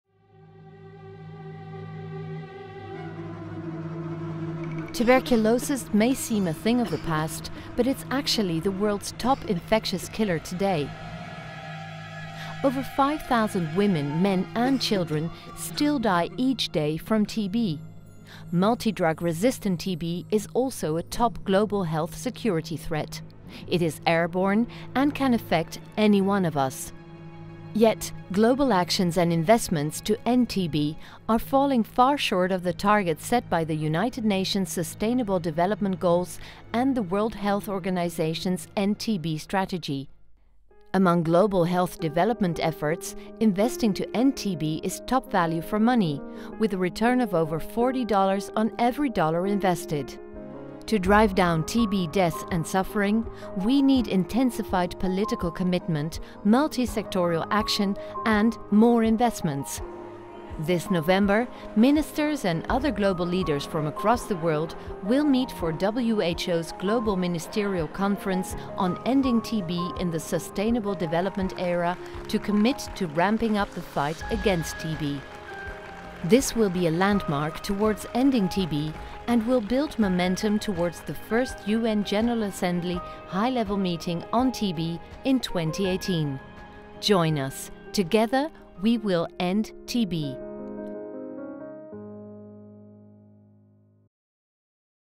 WHO: We will end Tuberculosis - Global Ministerial Conference on TB
Tuberculosis (TB) is the leading infectious killer in the world claiming 5,000 lives a day, and one of the top 10 causes of death worldwide. TB is preventable and curable, however, actions and investments fall far short of what is needed to end this epidemic.  To drive down the number of TB deaths and suffering, we need more political commitment, multisectoral action, more investments and research. This November, Ministers and other global leaders from across the world, will meet in Moscow to commit to end the global TB epidemic in line with the targets set by the WHO End TB Strategy and the UN Sustainable Development Goals.